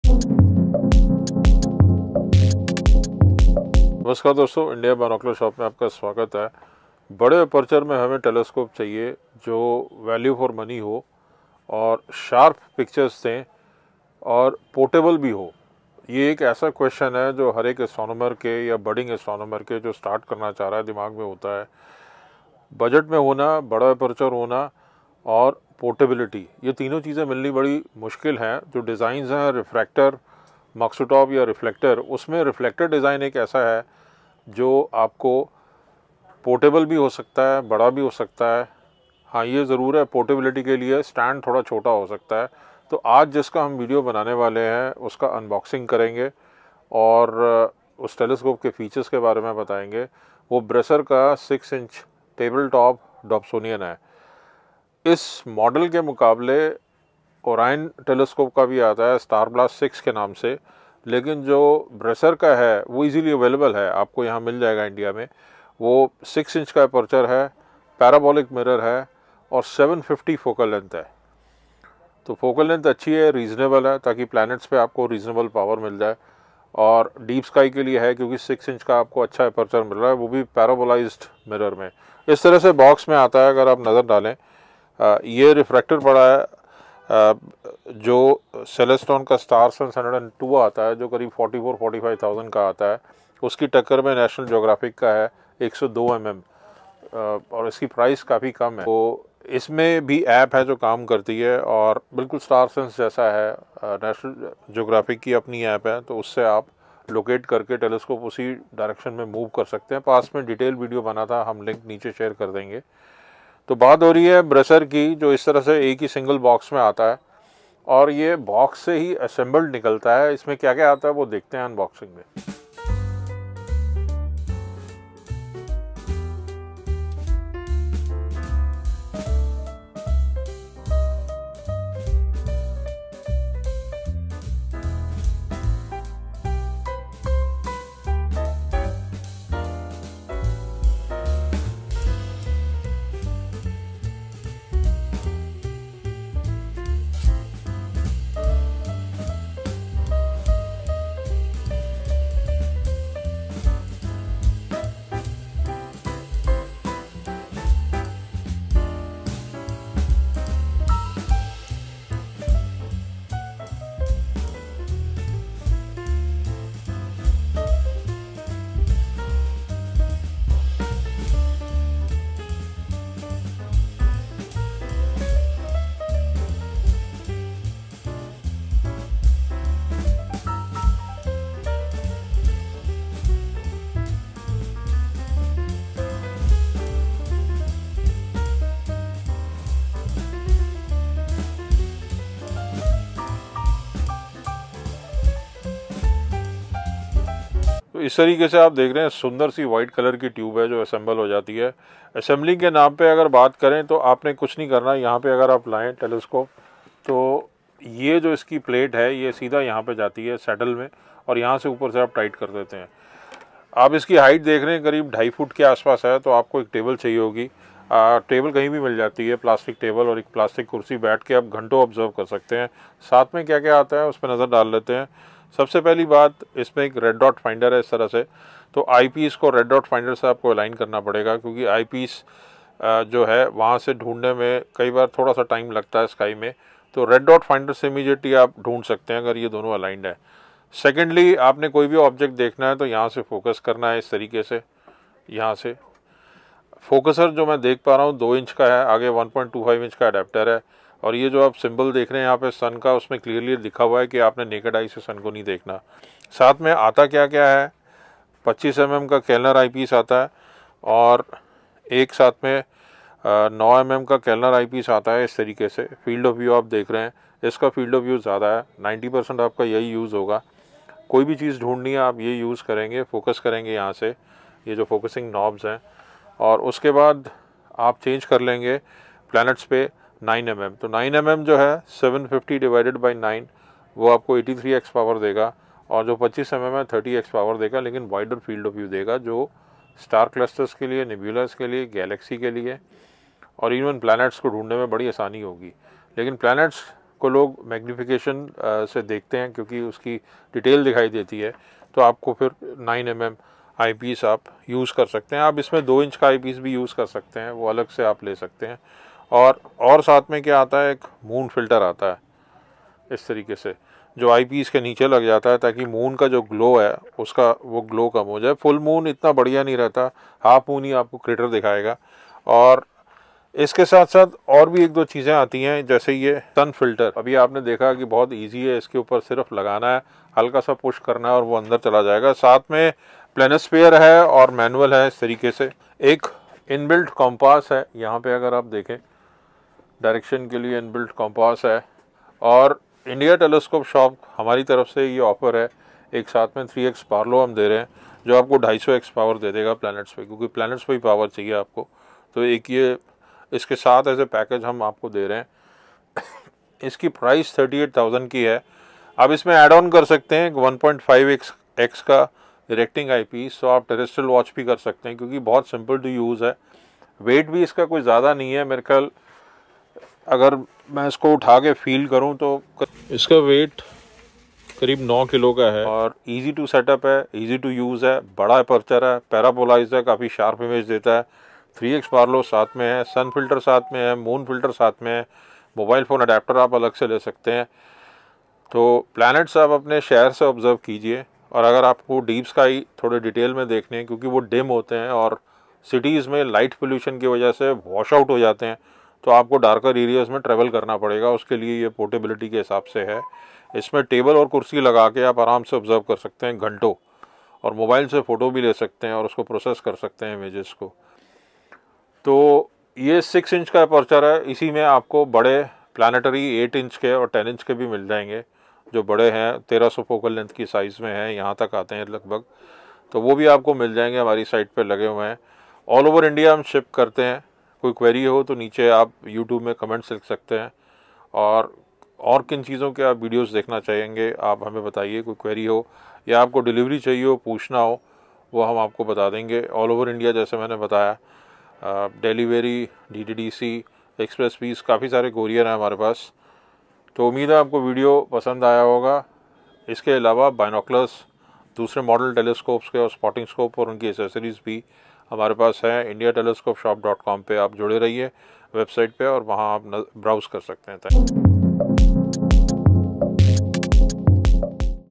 bresser Parabolic 6 inch 150/750mm Dobson telescope Unboxing,Features,Images all india ship-Hindi
bresser dobsonian 6 inch telescope free 3x barlow,sun filter , moon filter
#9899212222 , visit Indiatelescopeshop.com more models